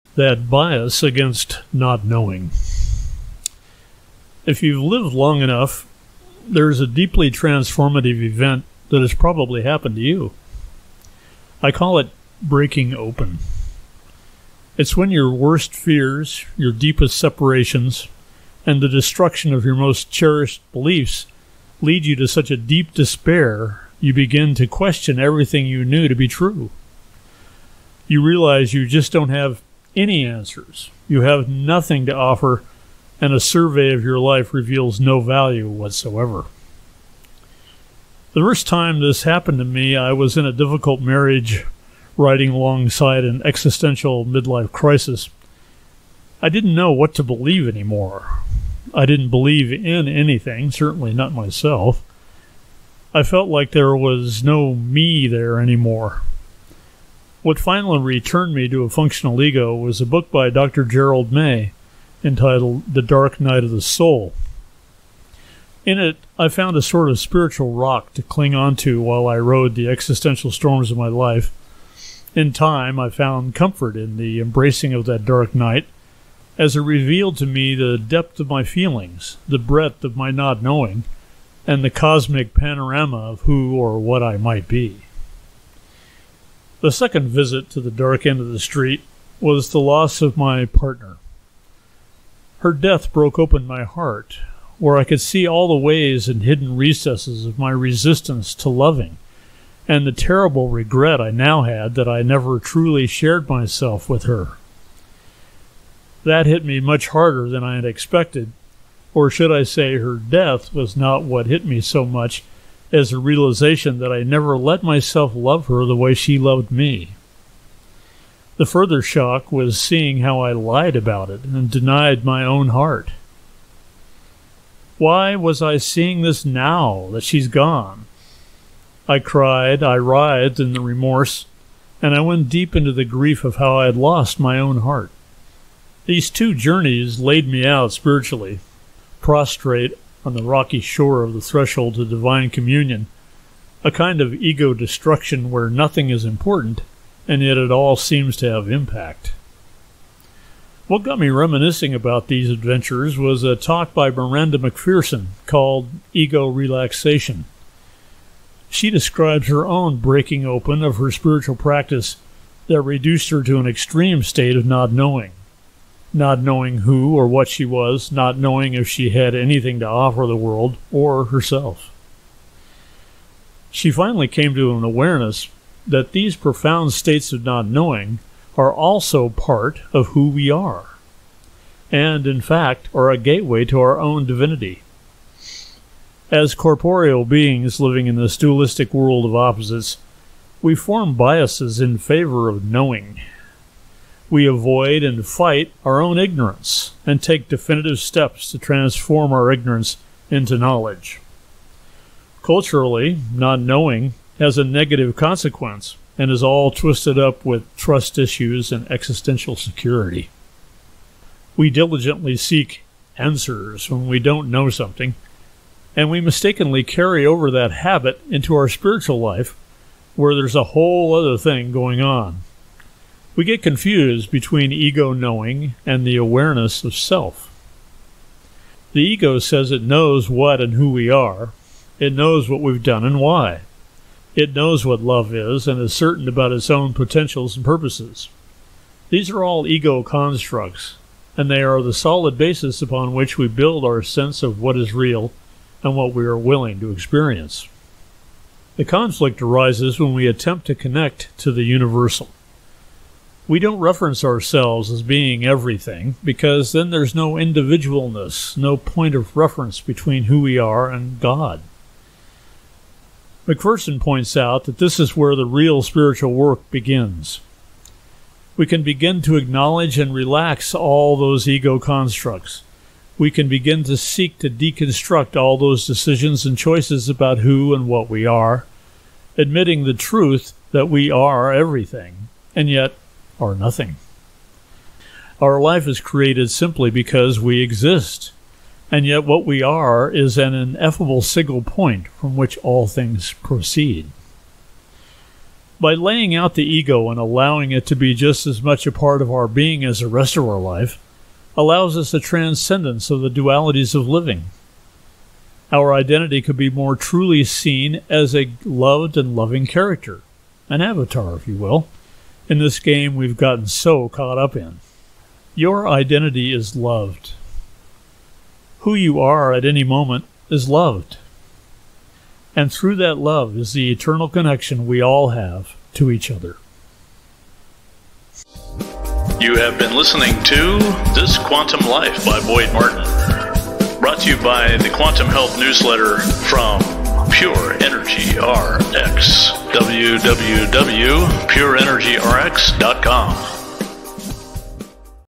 This Quantum Life #87 - That Bias Against Not Knowing (Ver. 2)
EXCERPT:  We diligently seek "answers" when we don't know something, and mistakenly carry over that habit into our spiritual life where there is a whole other thing going on. We get confused between ego knowing and the awareness of self.